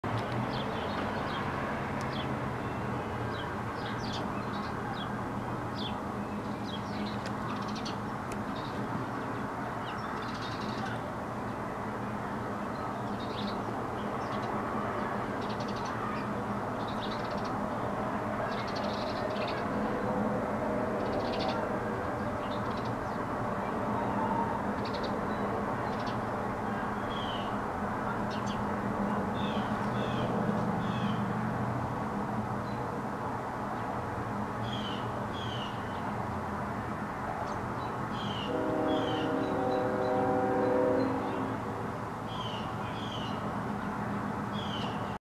Tashmere and the birds
Tashmere is excited that the window is open.